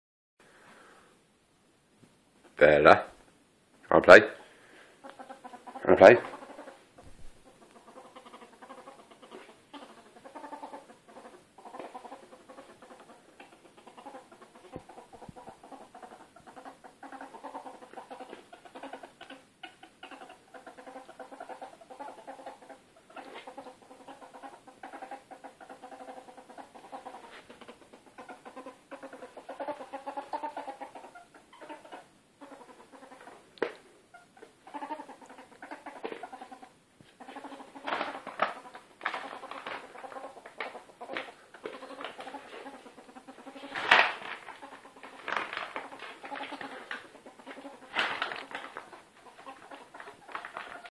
My ferret dooking
My ferret, Bella dooking while we play. Her dooking sounds very much like she is giggling. She could literally play and dook for hours. Sorry about the camera quality, just a quick video on my phone.

Bella was originally a hunting ferret before she was found and handed into the animal shelter, I took her home about two weeks ago and now she is a nice house ferret.